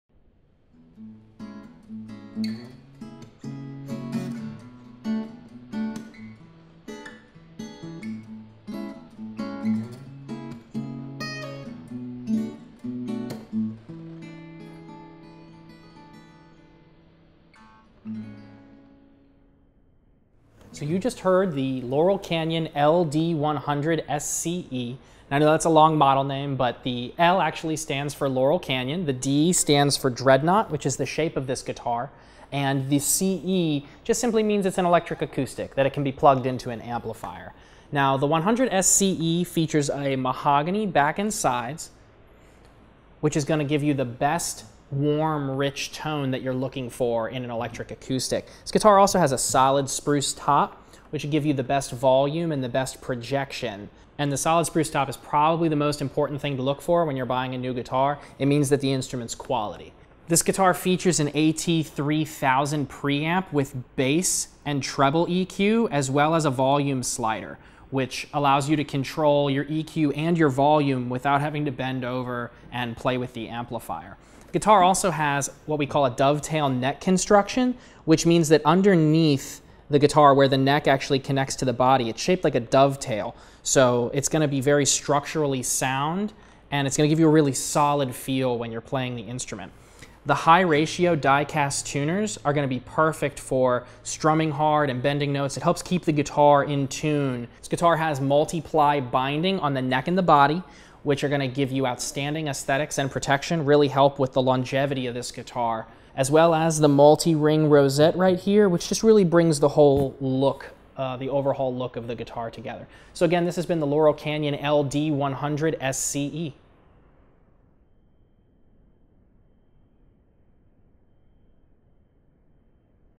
Laurel Canyon LD-100SCE Dreadnought
Item # 0196899 | Model # LD-100SCEHB

Well-crafted and delivering a warm rich tone, the Laurel Canyon LD-100SCE Dreadnought features a body constructed of a solid spruce top and mahogany on both the sides and back with a beautiful multi-ring rosette around the sound hole.

This acoustic/electric guitar also features multi-ply binding on the neck and body, dovetail neck joint and an AT3000 preamp that powers active electronics making the Laurel Canyon LD-100SCE Dreadnought ready to plug in and perform.

Features:

• Solid spruce top
• Mahogany on both the sides and back
• Powered by AT3000 preamp
• Beautiful multi-ring rosette
• Dovetail neck joint construction
• Multi-ply binding on the neck and body